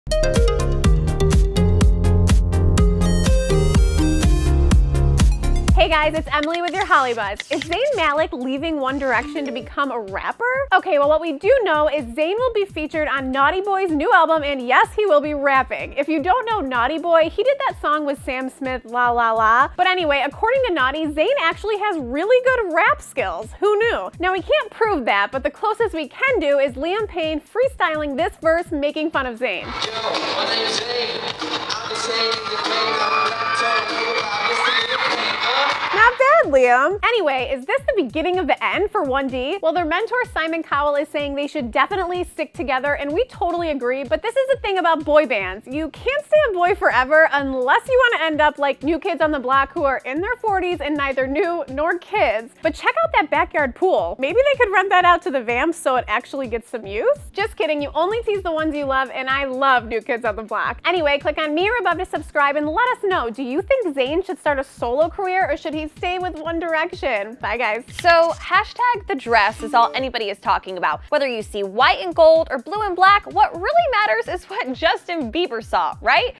Zayn Malik New Rap Career - Leaving One Direction?
Zayn Malik starting solo career to rap on Naughty Boy's new album. Is this the end of One Direction?

Starring Emily Longeretta